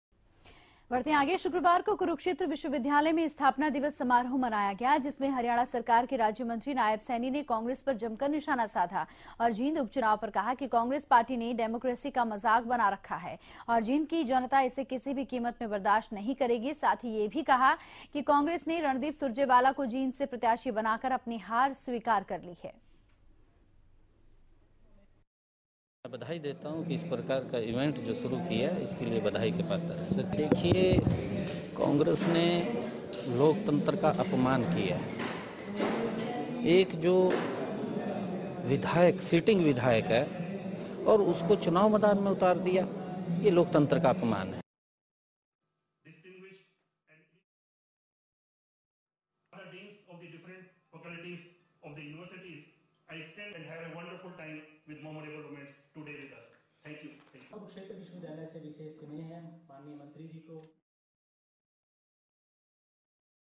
कुरुक्षेत्र विश्वविद्यालय में स्थापना दिवस समारोह
कुरुक्षेत्र विश्वविद्यालय में स्थापना दिवस समारोह मनाया गया. जिसमें हरियाणा सरकार के राज्यमंत्री नायब सैनी ने कांग्रेस पर जमकर निशाना साधा और जींद उपचुनाव पर कहा कि कांग्रेस पार्टी ने डेमोक्रेसी का मजाक बना रखा है.